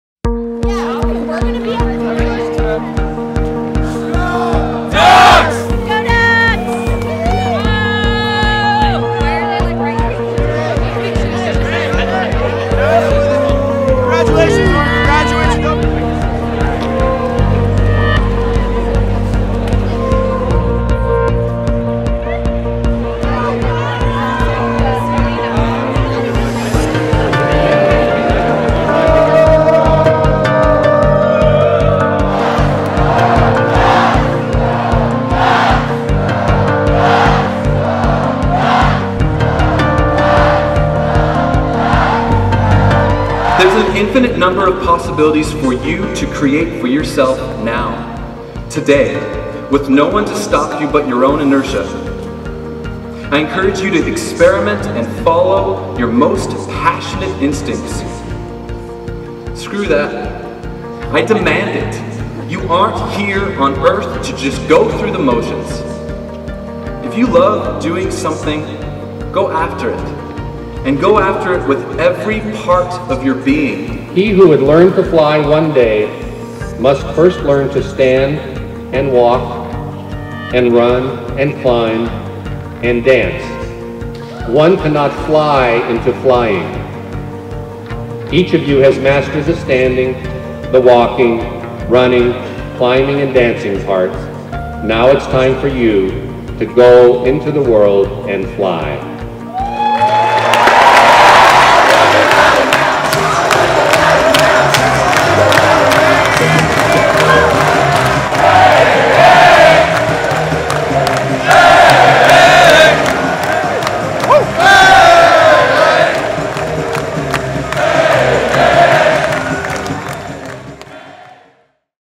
2015 Commencement Ceremony
Scenes from the 2015 Spring commencement ceremony on Monday, June 15, 2015, on the campus of the University of Oregon in Eugene, Oregon.